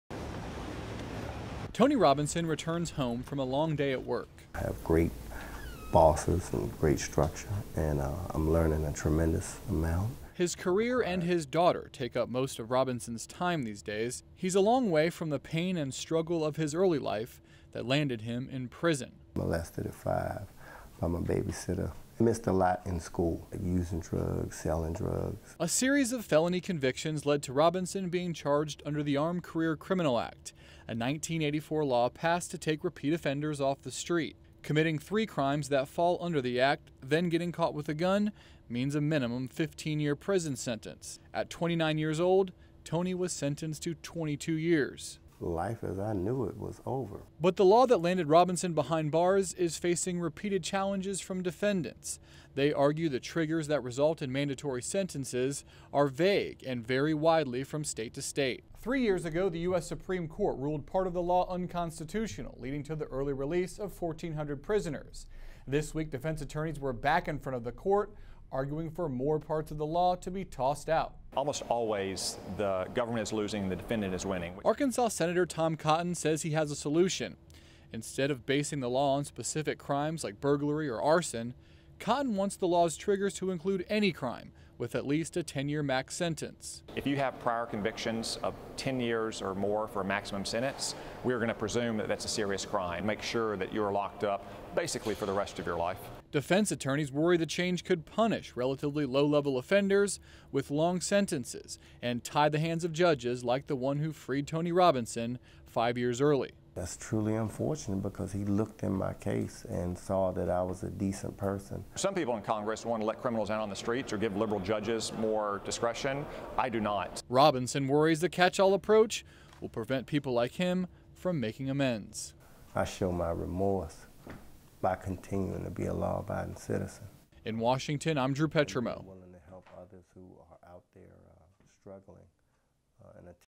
Challenge to federal prison sentencing law argued before Supreme Court
The U.S. Supreme Court this week heard another challenge to a decades old federal that applies tough prison sentences for certain repeat offenders.

The court already invalidated part of the law over concerns that it's too vague.

Arkansas Senator Tom Cotton says he has a solution. Instead of basing the law on specific crimes like burglary or arson, Cotton wants the law's triggers to include any crime with at least a ten year max sentence.

Defense attorneys worry the change could punish relatively low level offenders with long sentences and tie the hands of judges like the one who freed tony robinson five years early.